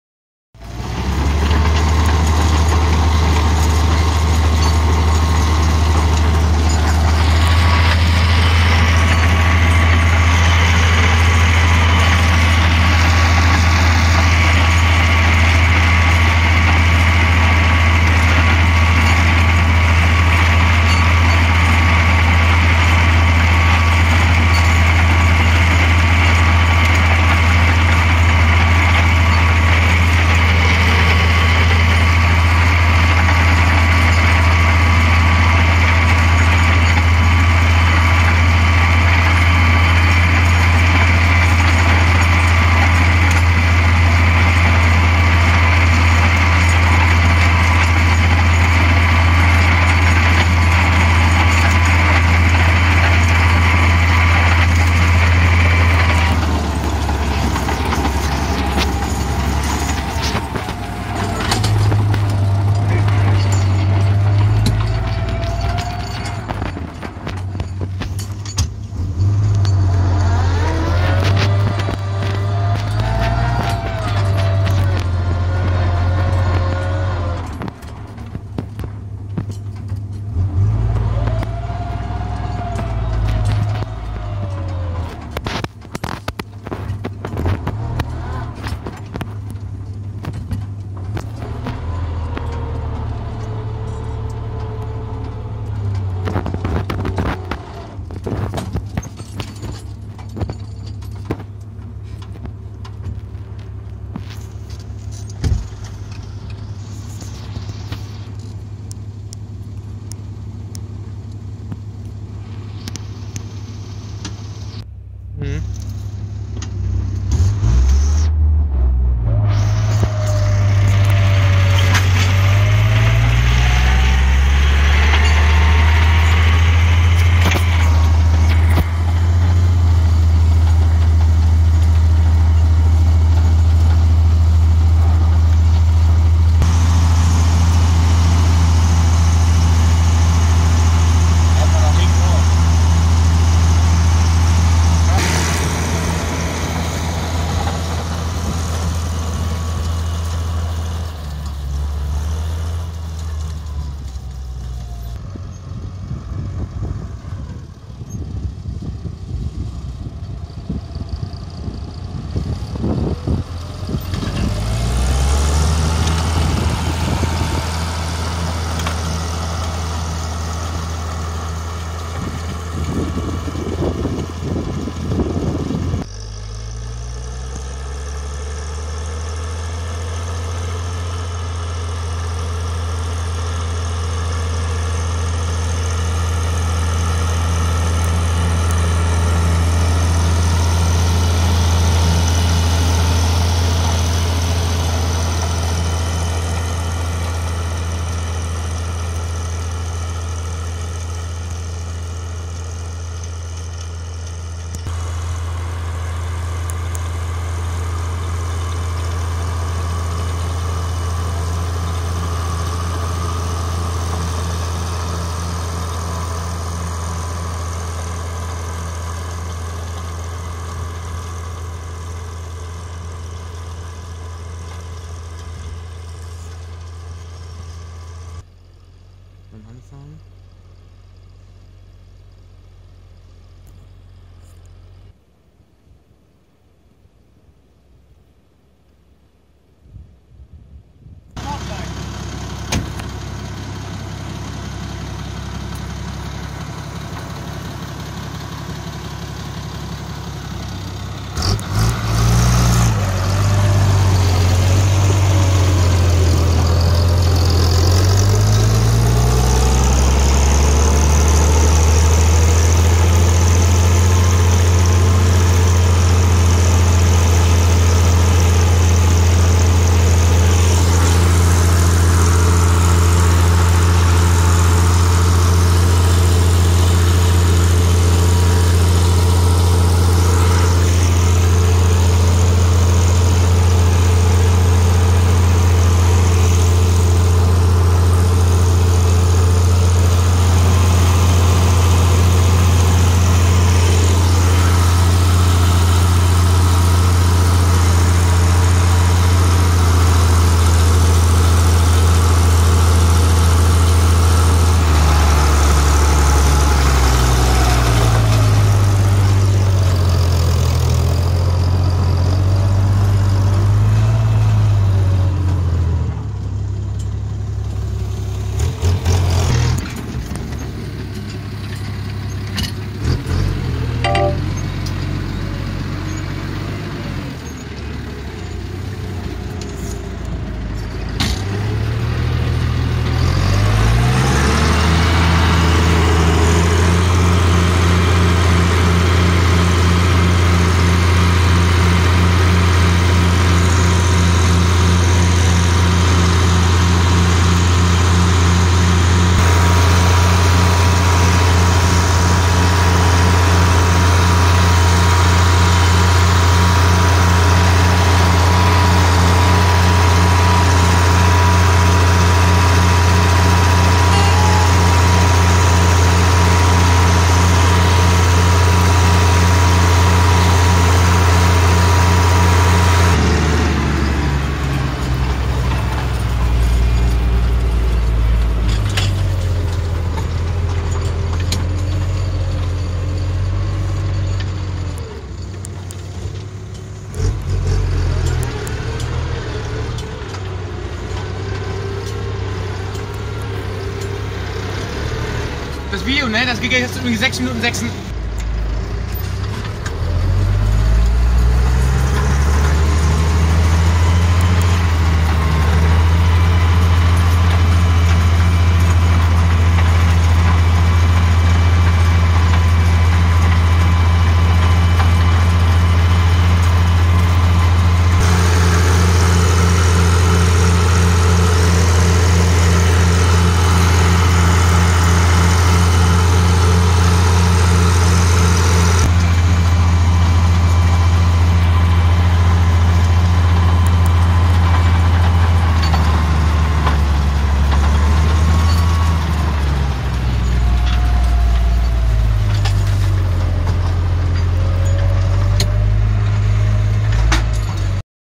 Am säen mit IHC 956 mit Drillmaschine
Es wurde mit einer 3m Niemeyer Kreiselegge und 3m Sämaschine von Amanoze D7 Typ 30 Triticale gesät